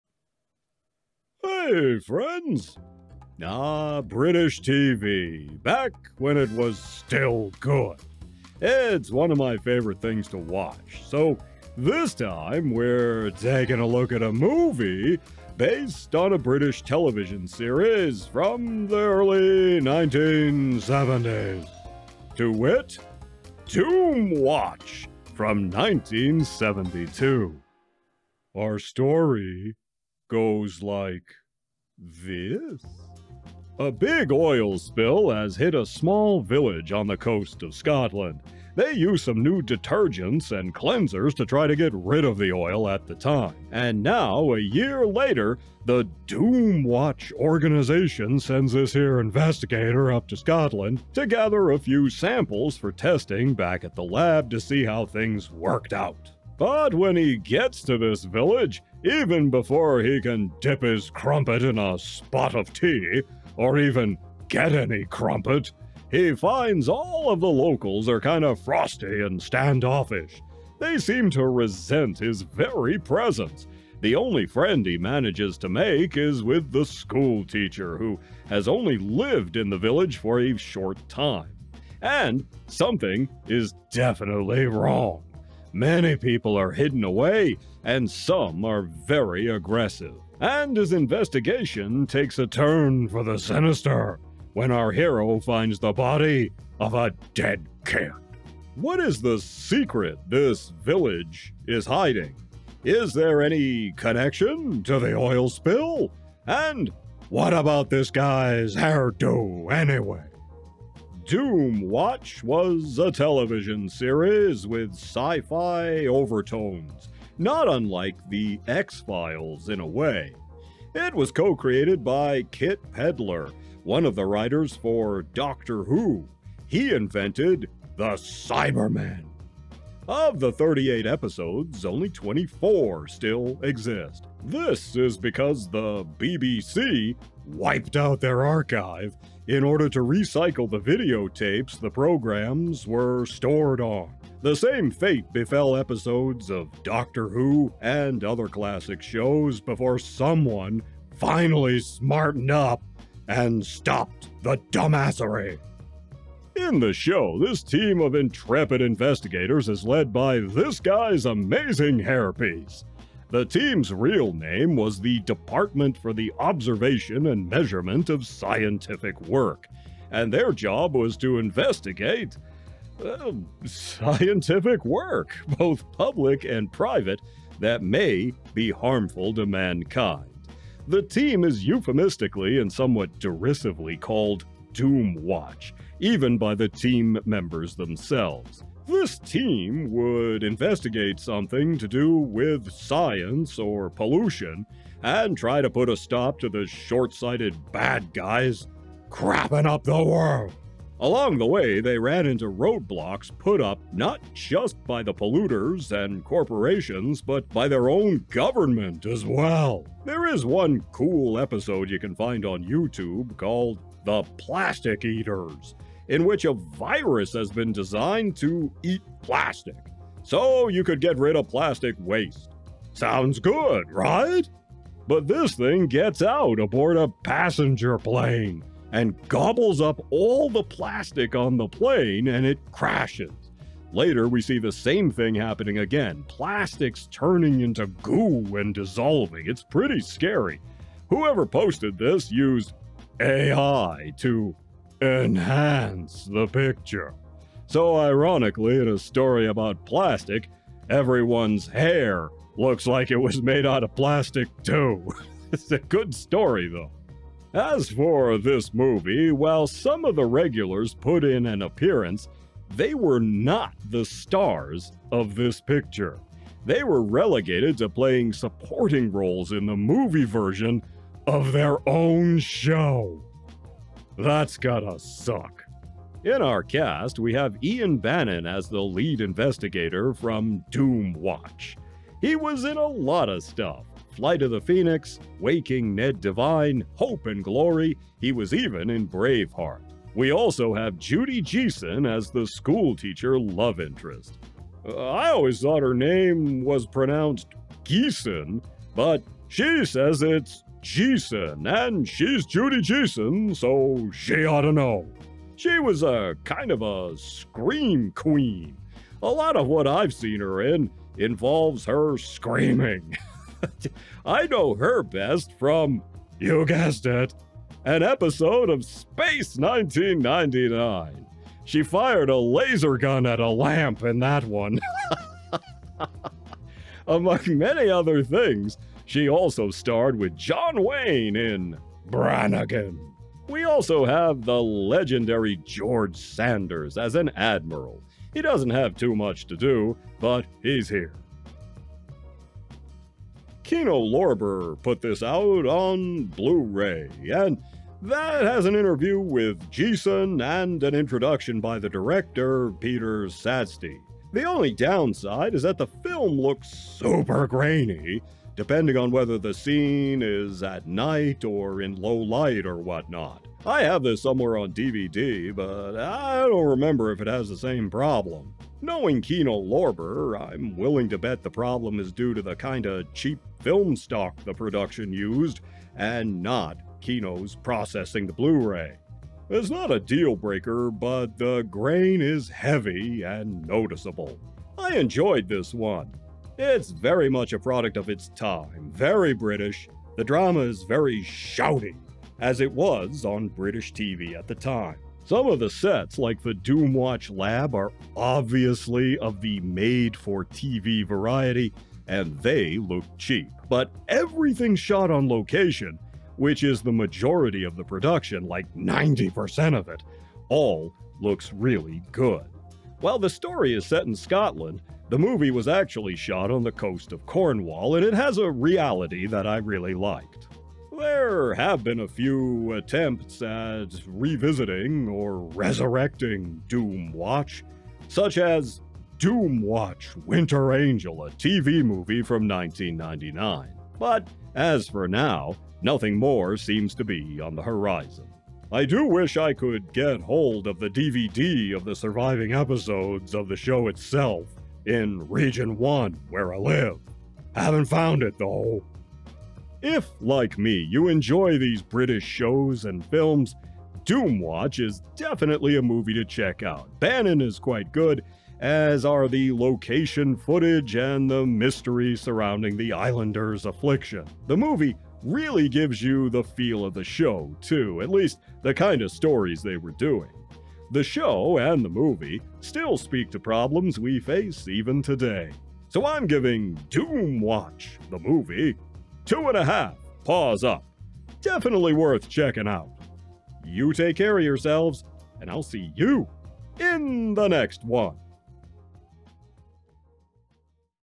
70's Style Eco-Action! Amazing Hair! "Doomwatch" (1972) Movie Review
In this movie review, the Barkster explores the world of "Doomwatch" (1972), a movie based on a British TV show of the same name. In this one, an investigator for an ecological government agency runs into mystery and danger when he visits a small coastal village previously hit by an oil spill the year before.  Ian Bannen and Judy Geeson star, while the cast of the TV show are relegated to playing second fiddle. George Sanders and Geoffrey Keen pop up in small supporting roles.

00:00 British TV
00:28 What is "Doomwatch" (1972) about?
01:52 "Doomwatch" the TV series
03:30 "The Plastic Eaters"
04:19 The cast of "Doomwatch" (1972)
05:43 The "Doomwatch" (1972) Blu-ray from Kino Lorber
06:24 Location vs. Studio
07:00 "Doomwatch: Winter Angel" (1999)
07:18 Barkey's score and final thoughts

Barkey's Wish List